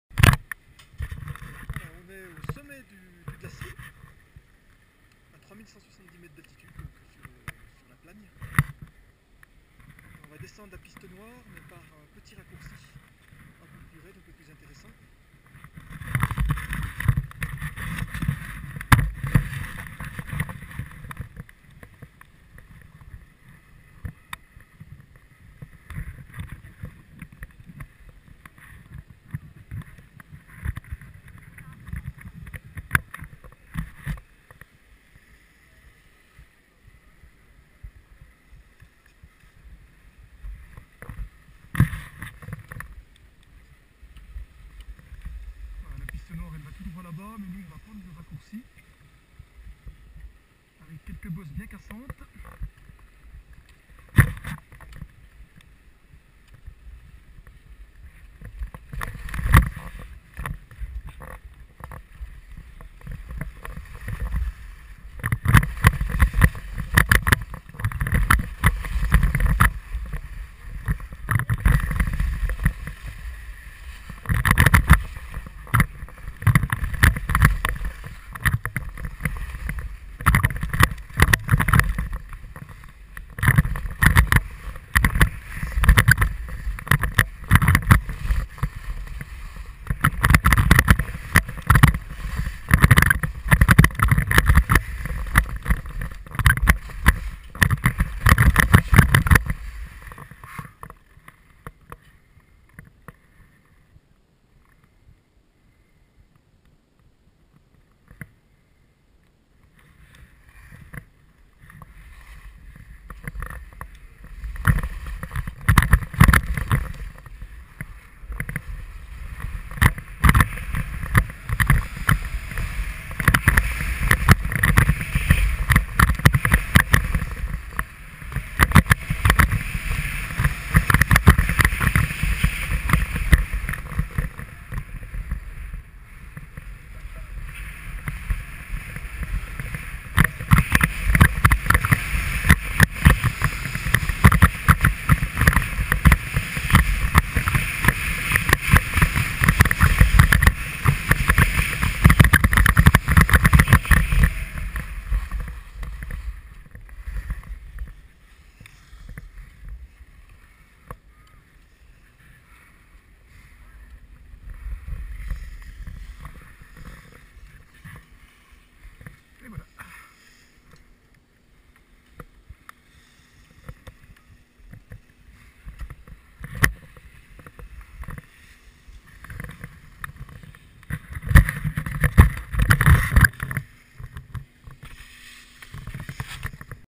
La Plagne. Piste noire du télésiège du Glacier. 19-03-2015
ANNEE : 2015
MOIS : 03
JOUR : 19

LIEU : LA PLAGNE
Piste noire du télésiège du Glacier
Départ à 3170 m d'altitude

EVENEMENT : Séjour de ski à Montchavin
Descente avec la Go Pro

PERSONNAGE(S) : moi